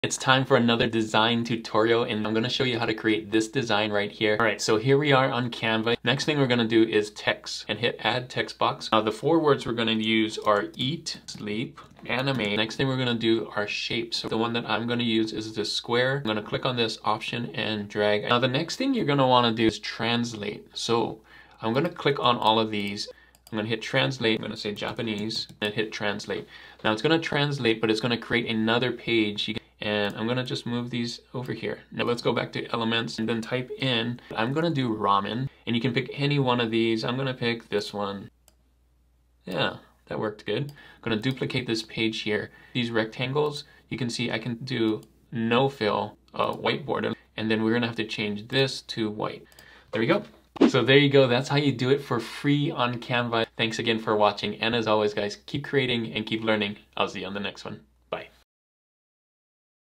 Eat Sleep Anime Repeat T-Shirt Design Tutorial in Canva
Here is how to use Canva to create a cool t-shirt design. This is the Eat Sleep Anime Repeat design with Japanese text.

Elgato Key Light
LG 38" UltraWide Monitor